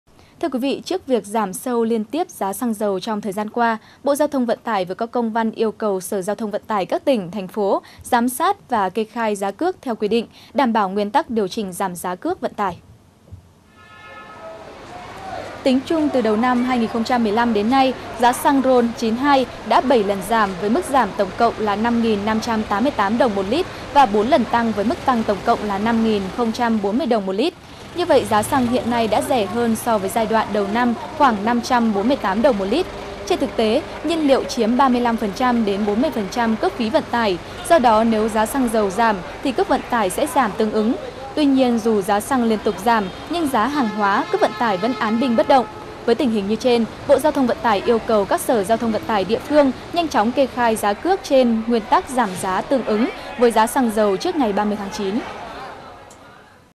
VTC14_Bộ GTVT yêu cầu giảm giá cước vận tải trước 30/9
(Truyền hình VTC14) - Trước việc giảm sâu liên tiếp giá xăng dầu trong thời gian qua, Bộ Giao thông vận tải vừa có công văn yêu cầu Sở Giao thông vận tải các tỉnh, thành phố giám sát và kê khai giá cước theo quy định, đảm bảo nguyên tắc điều chỉnh giảm giá cước vận tải.